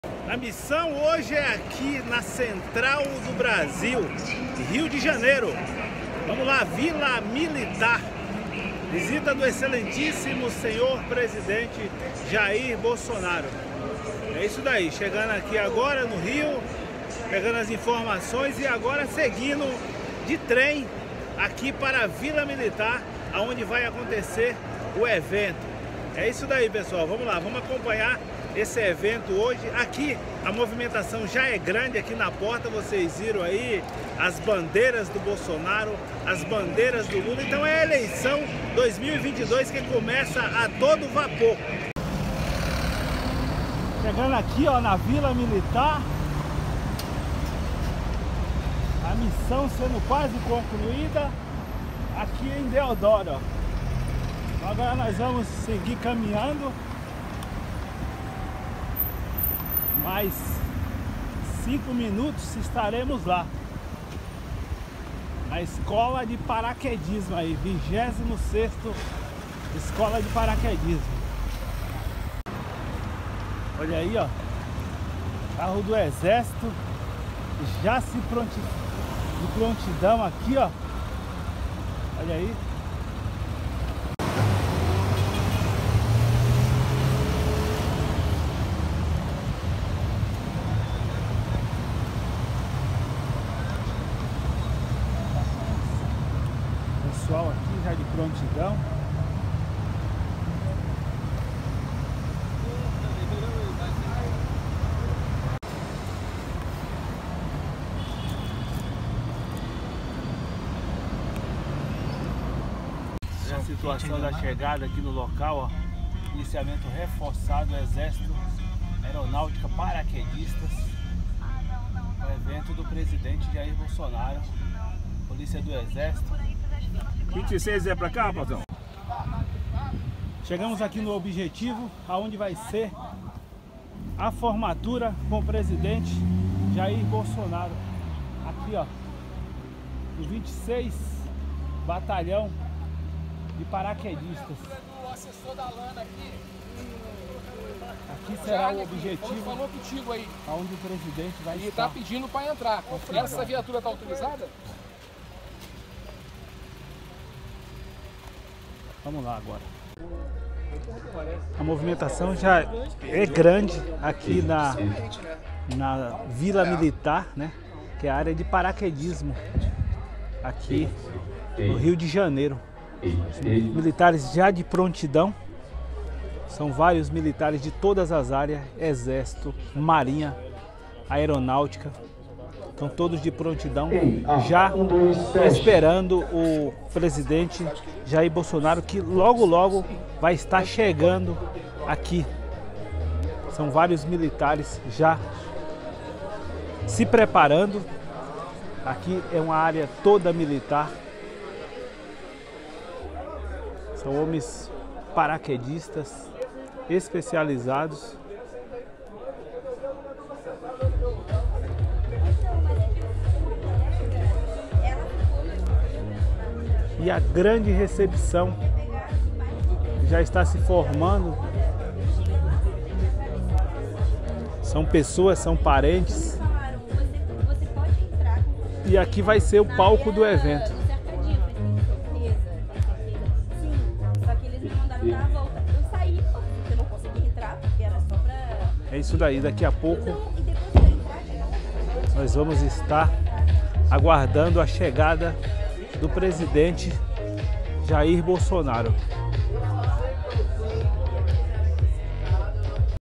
Bolsonaro na Formação Paraquedistas do 26 BIpqdt
Presidente Jair Bolsonaro Participa de Formação de Para Quedistas no Rio de Janeiro na Vila Militar